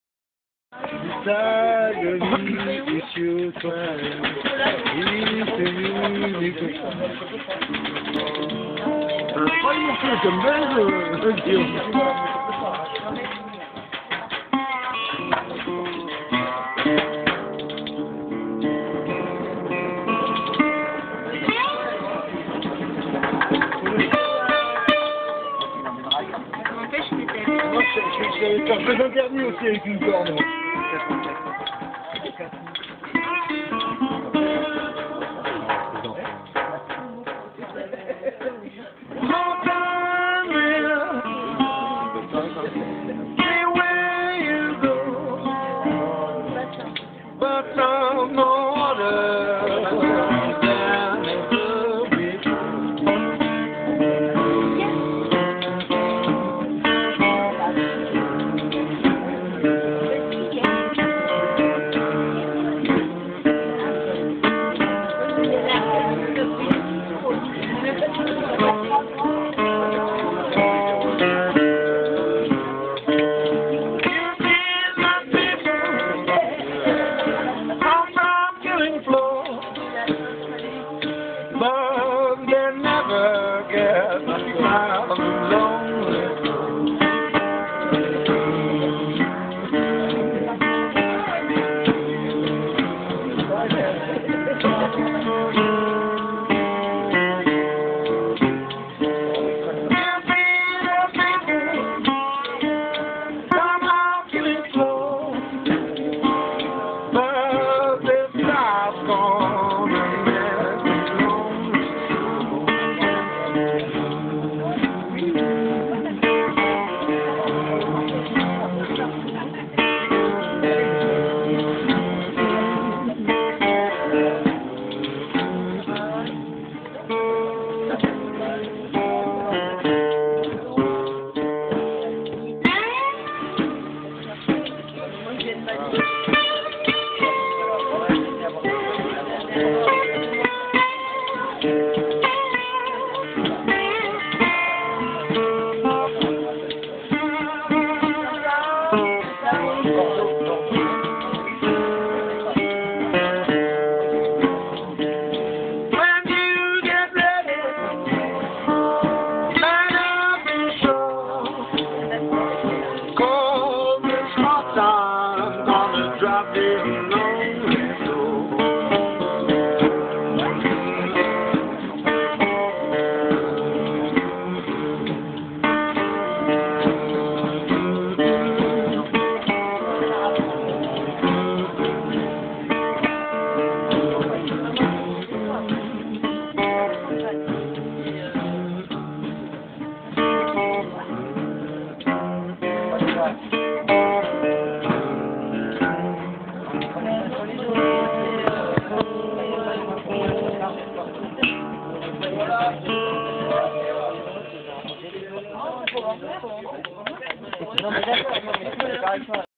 Bone Tee & The Slughunters
Fin de soirée à Au Bouchon à Cazaux...